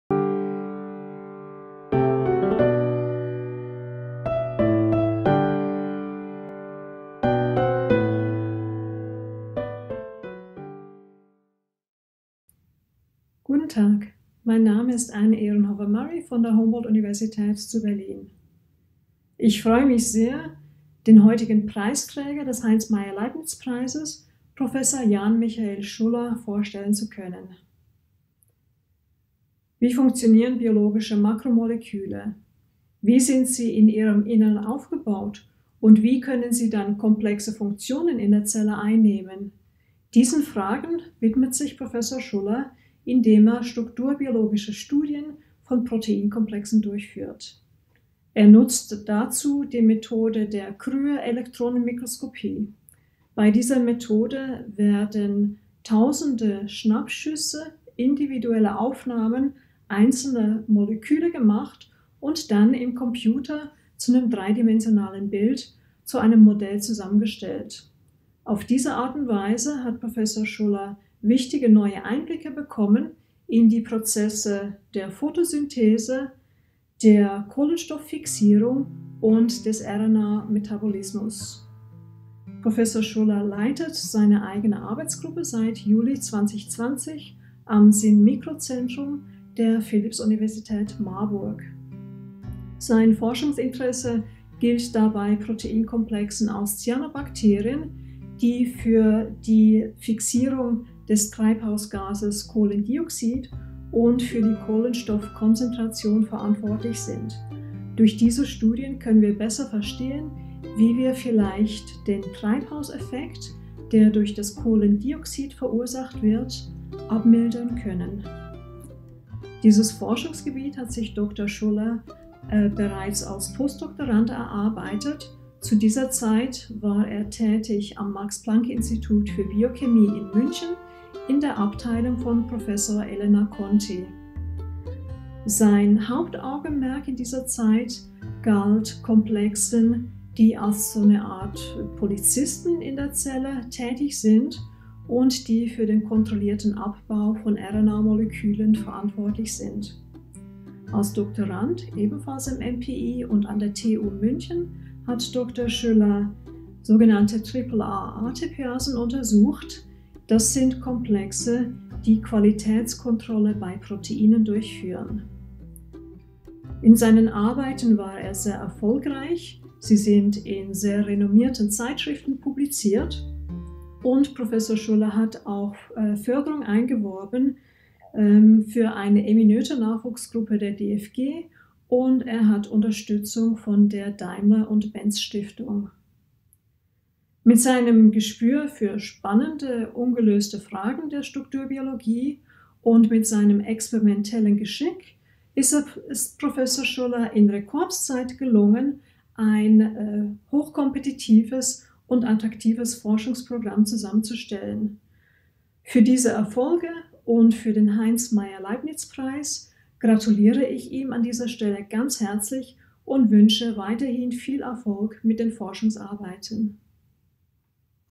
Jan Michael Schuller - Heinz Maier-Leibnitz-Preisträger 2021
Biochemie und Biophysik der Mikroorganismen, Philipps-Universität Marburg

Jan Michael Schullers Forschung hat auf verschiedenen Gebieten der Strukturbiologie, Biophysik und Biochemie neue Einblicke in grundlegende biologische Mechanismen hervorgebracht. So trug Schuller maßgeblich zum tieferen Verständnis der Funktion photosynthetischer Komplexe bei Cyanobakterien bei, was auch relevant in Bezug auf Anwendungsaspekte ist. Mit seiner von der DFG geförderten Emmy Noether-Nachwuchsgruppe untersucht Schuller in Cyanobakterien die Struktur von Enzymen, die an der Kohlenstofffixierung und dem Kohlenstoffkonzentrationsmechanismus beteiligt sind. Cyanobakterien sind photosynthetische Bakterien, die beispielsweise einen hohen Anteil im marinen Phytoplankton ausmachen. Schuller konnte an diesen Organismen zeigen, auf welche Weise die Kohlenstoffkonzentration durch das Wechselspiel des für die Fixierung von CO2 aus der Atmosphäre verantwortlichen Enzyms Rubisco und einer Kohlensäureanhydrase, die für CO2-Nachschub sorgt, erfolgt. Möglich wurde die Aufklärung dieses Mechanismus durch Schullers innovative strukturbiologische Methoden wie die Anwendung der Kryoelektronenmikroskopie auf Einzelmolekülebene.